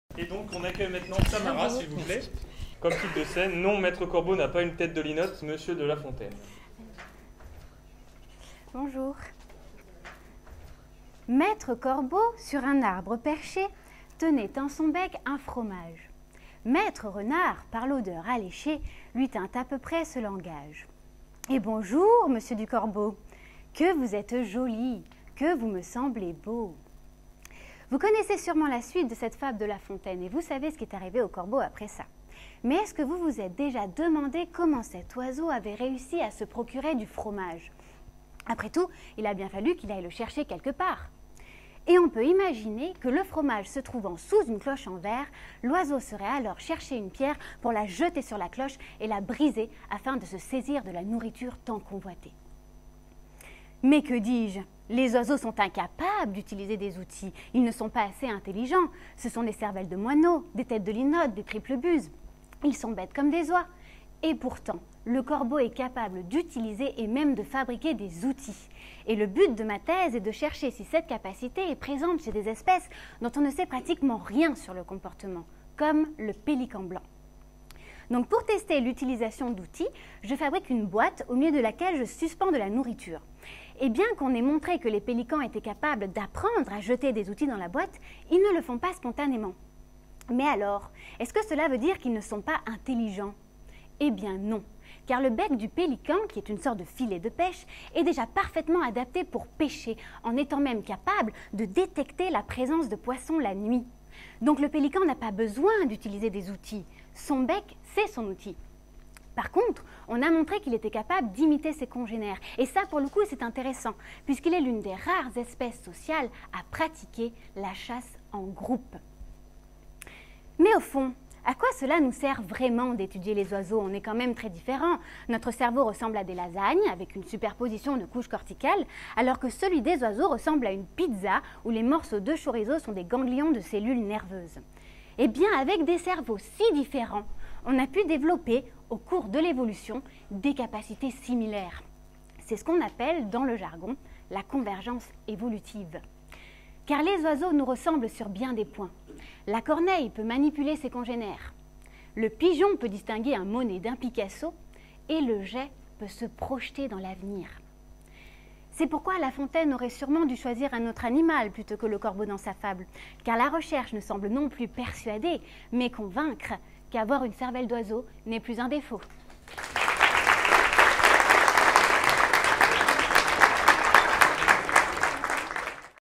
MT180Lyon Samara Danel : « Non, Maître Corbeau n'est pas une tête de linotte ! »
Qualifiée pour la finale lyonnaise de l'édition 2018 du concours Ma thèse en 180 secondes, Samara Danel, doctorante au laboratoire d'étude des mécanismes cognitifs (Université Lumière-Lyon 2) nous montre que les oiseaux sont loin d'être stupides…

Son sujet de thèse : « Cognition physique chez l’oiseau : générale ou adaptée ? »